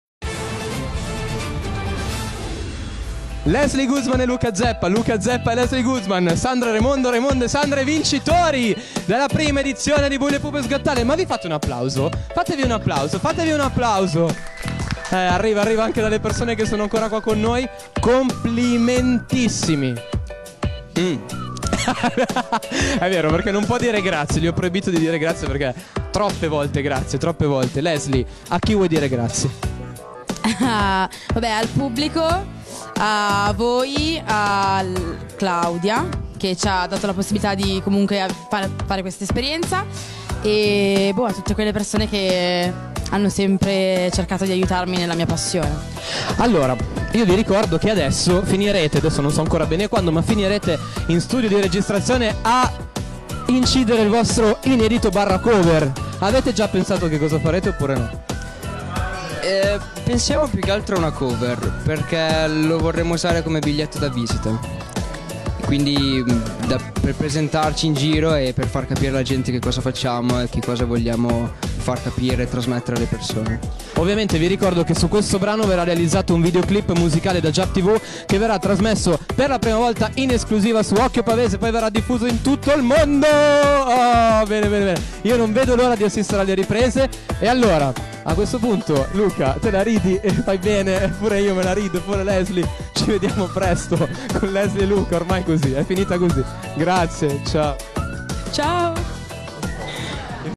Leslie & Luca vincono la 1ª stagione di Bulli e Pupe
Intervista di Marco Cignoli